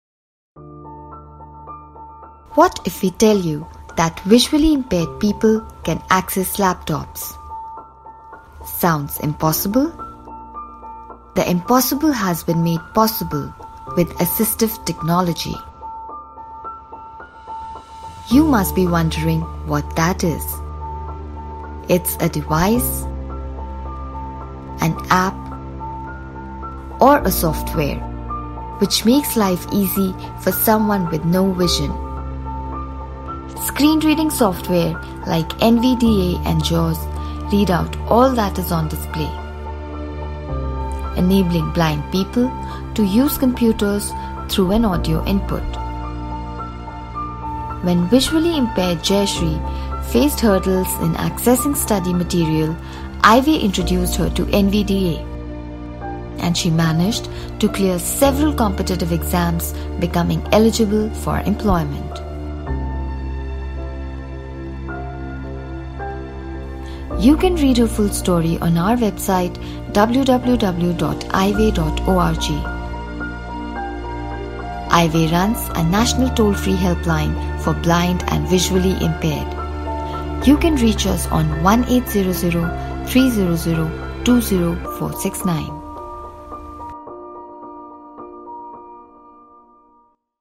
Assistive Technology for visually impaired
Very often a small solution can make a huge difference in someone’s life. In the case of blind and visually impaired people who don’t have access to a host of mainstream activities, a simple technological solution can bridge the otherwise vast gap!

Watch this video to find out how accessing a laptop has been made possible through ‘Assistive Technology’.